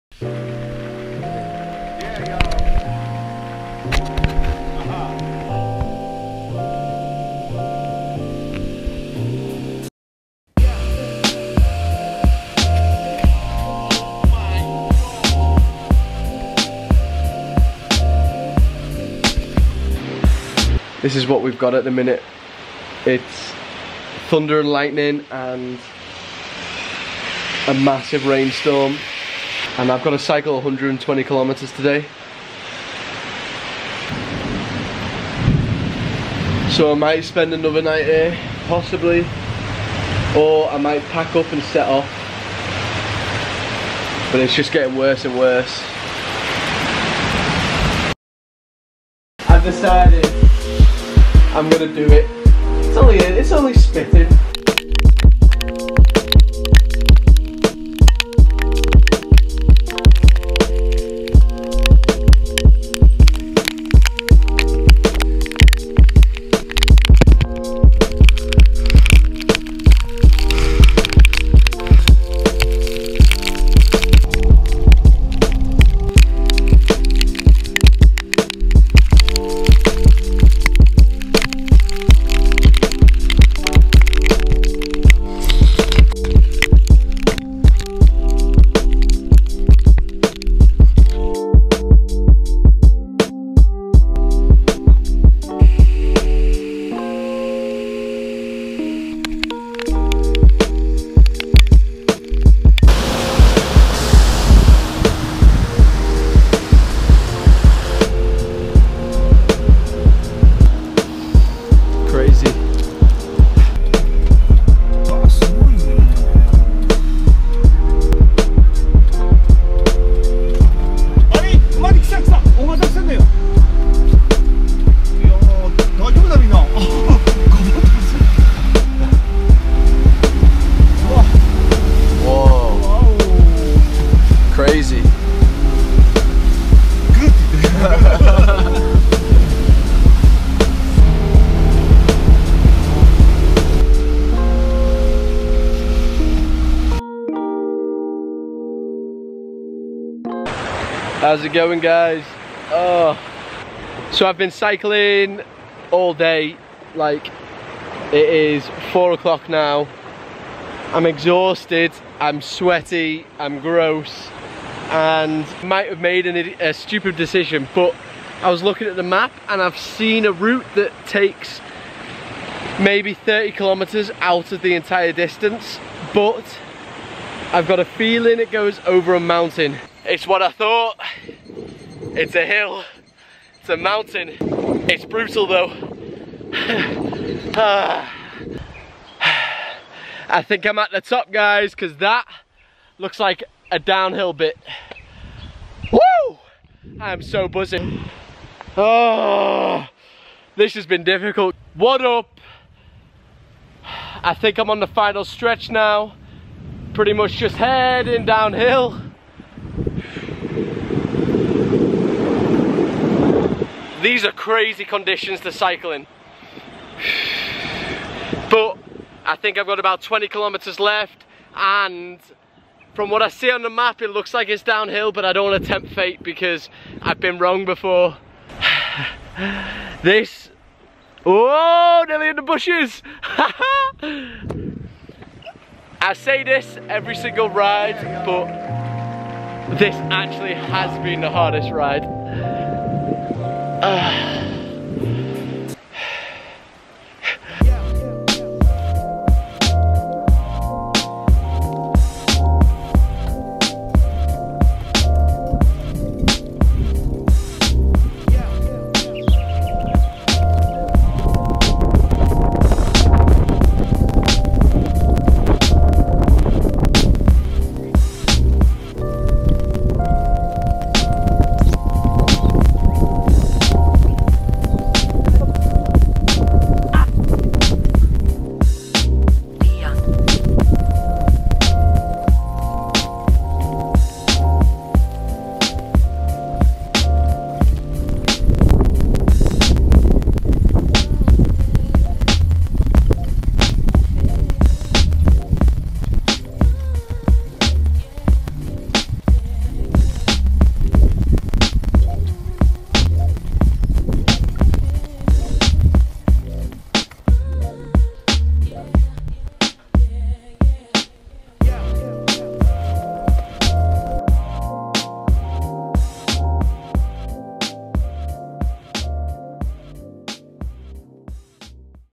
THE JAPAN DIARY - ENTRY 23 - 'IWATE FLOODS'
Stories of an Englishman cycling Japan end to end.
Filmed & Edited - Peter Richards